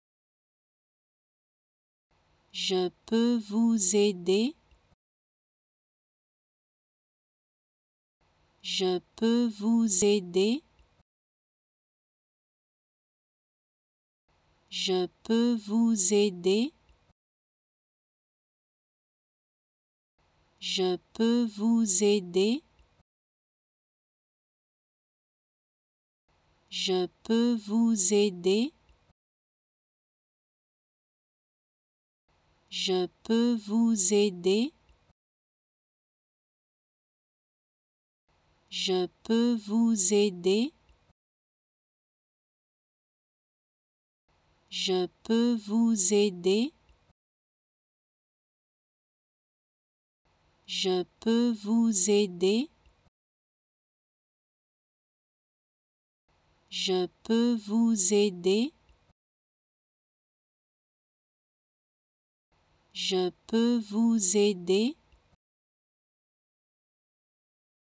How to Pronounce ''Je peux vous aider'' (Can I help you?) Correctly in French
Learn how to say and properly pronounce ''Je peux vous aider'' in French with this free pronunciation tutorial. Apprenez à prononcer des mots en français grâce à nos tutoriels.